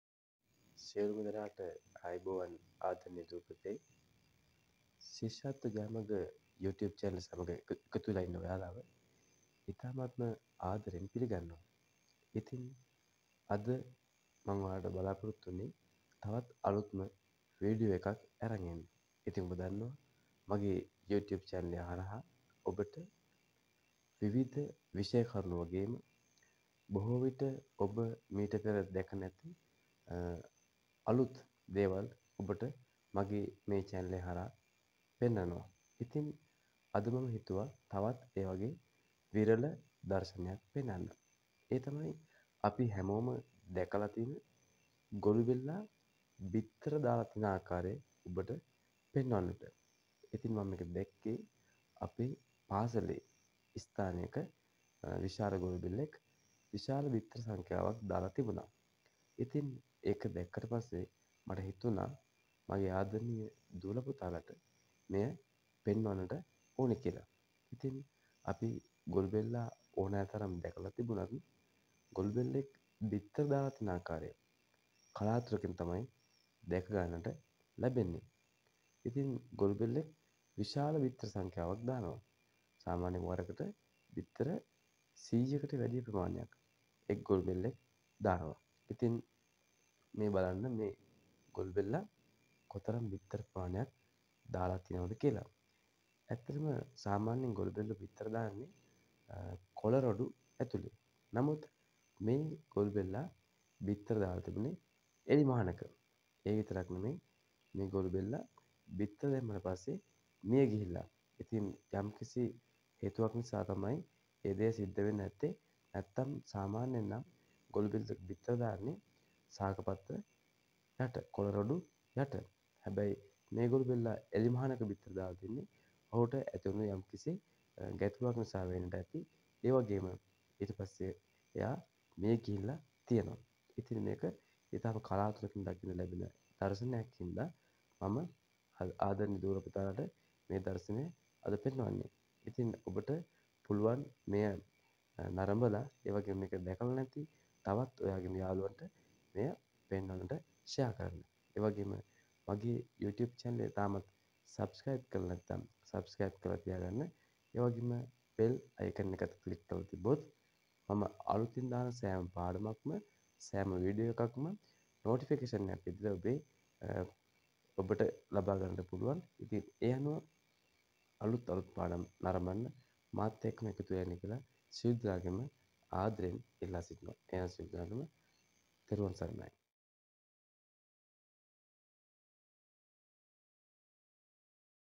ගොළුබෙලි බිත්තර ශිෂ්‍යත්ව ජය මඟ ප්‍රසන්න සර් prasanna sir
ඉතා කලාතුරකින් දකින්නට ලැබෙන දර්ශනයක්.පරිසරය සමඟ ගනුදෙනු කිරීමේ දී අපට මෙවැනි බොහෝ දෑ දැකගත හැකියි.